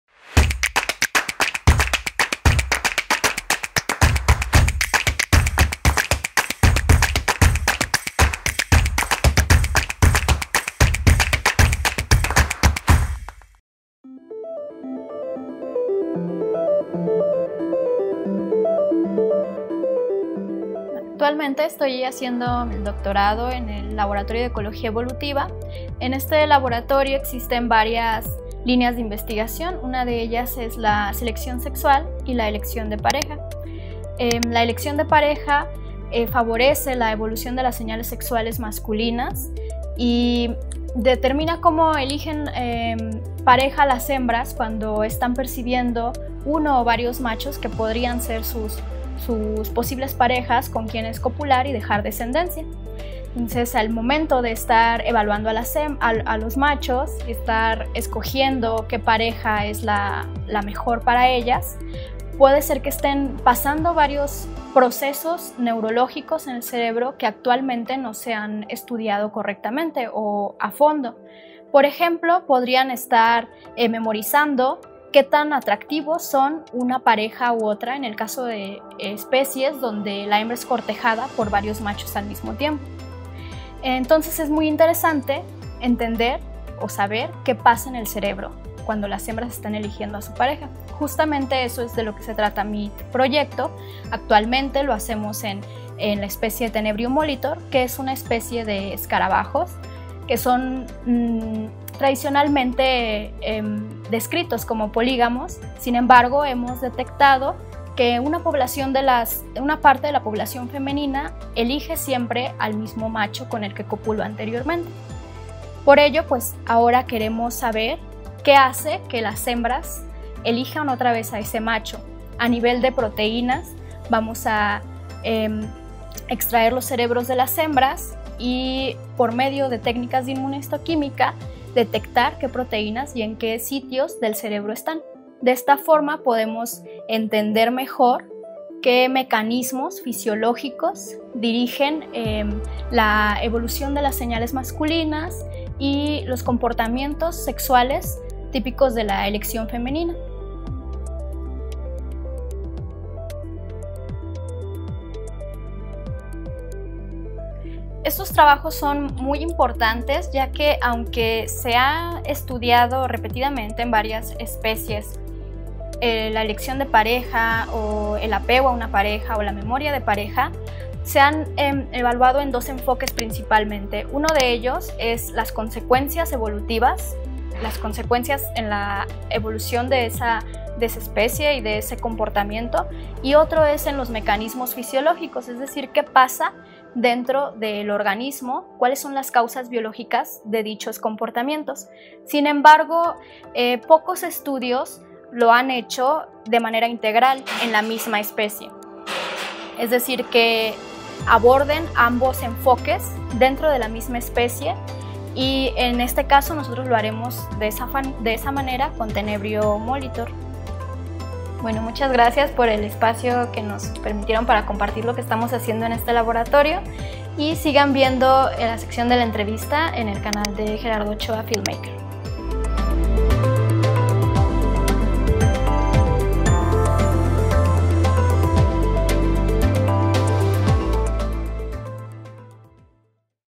La importancia de la memoria femenina en la selección sexual (caso Tenebrio molitor)
Entrevista con la Dra. Sagrario Cordero Molina del Laboratorio de Ecología Evolutiva de la ENES, UNAM Campus Morelia. Hablamos sobre los procesos neurológicos que influyen en la memoria femenina (en esta caso en escarabajos de la especie Tenebrio molitor) al momento de la selección de pareja.